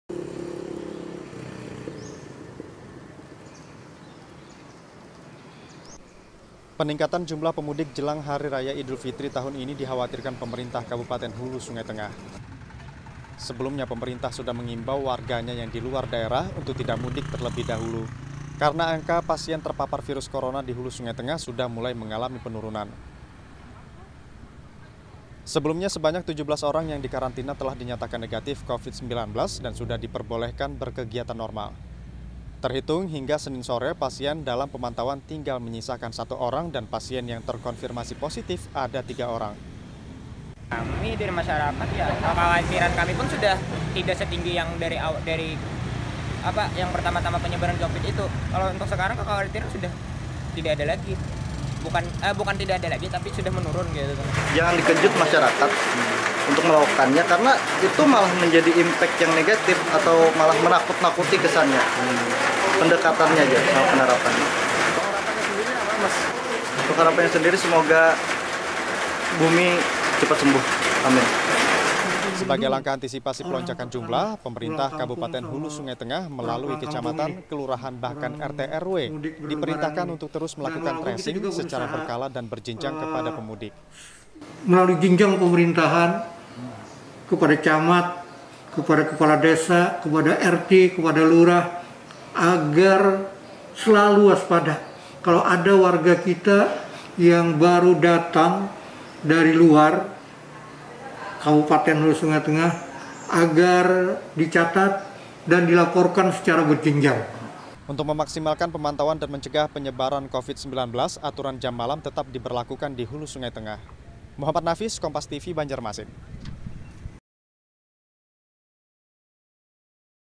Pemkab Hulu Sungai Tengah Waspadai Masuknya Covid-19 dari Arus Mudik
BARABAI, HST, KOMPAS.TV - Peningkatan jumlah pemudik jelang Hari Raya Idul Fitri tahun ini, dikhawatirkan pemerintah Kabupaten Hulu Sungai Tengah.
Sebelumnya pemerintah sudah menghimbau warganya yang diluar daerah untuk tidak mudik terlebih dahulu, karena angka pasien terpapar virus korona di HST sudah mulai mengalami penurunan.

Sebelumnya sebanyak 17 orang yang dikarantina telah dinyatakan negatif covid 19 dan sudah diperbolehkan berkegiatan normal.

Terhitung hingga senin sore pasien dalam pemantauan tinggal menyisakan 1 orang dan pasien yang terkonfirmasi  positif ada 3 orang.

Sebagai langkah antisipasi pelonjakan jumlah, pemkab HST melalui kecamatan kelurahan bahkan RT RW diperintahkan untuk terus melakukan tracing secara berkala dan berjenjang kepada pemudik.

Untuk memaksimalkan pemantauan dan mencegah penyebaran virus korona   aturan jam malam tetap di berlakukan  di hulu sungai tengah